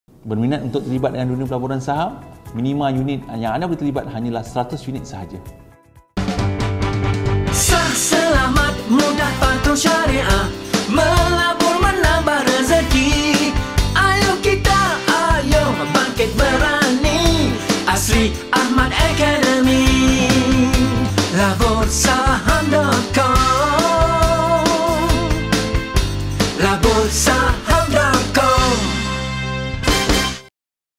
Berapa Minimum Unit Nak Beli Saham?
Berminat nak mula melabur saham?

Nak tahu tak, minimum unit untuk kita melabur saham hanya 100 unit atau juga dikenali sebagai 1 lot.

Katakan kita nak beli saham $Topglove, seunit: RM27.86

Minimum lot pembelian ialah 100 unit jadi jumlah untuk dibayar = RM2,786.00 (tidak termasuk kos brokerage dll)

Kalau nak contoh saham harga marhaen, saham $FPGroup, seunit: RM1.17

Minimum lot pembelian ialah 100 unit jadi jumlah untuk dibayar ialah = RM117.00 (tidak termasuk kos brokerage dll)

Ini baru satu contoh saya beri, di Bursa Malaysia ada beratus-ratus saham yang berharga RM1 ke bawah, maka modal minima untuk melabur tak sampai pun RM100 pun boleh.

Yang penting, kena pandai pilih saham yang baik.

Jika anda "Zero" tentang pelaburan saham di Bursa Malaysia sertai kami di